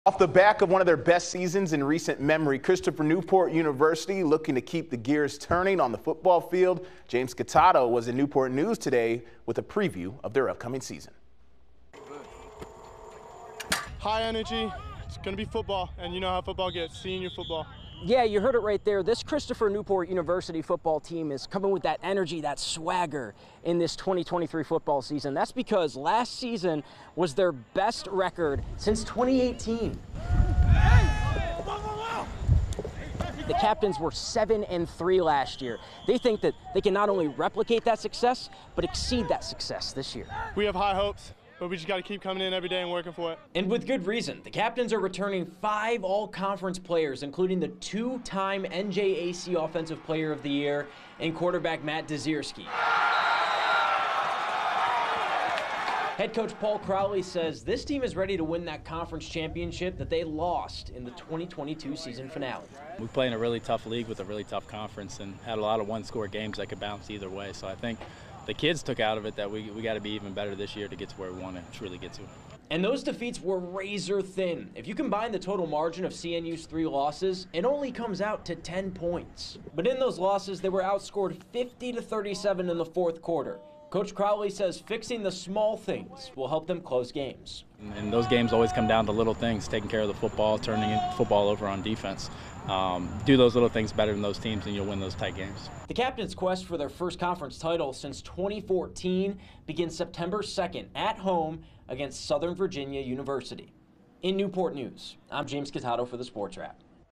CNU Football looks to build on best season in recent memory
CNU Football went 7-3 in 2022, which was their best season in years. They want to continue that momentum heading into the 2023 campaign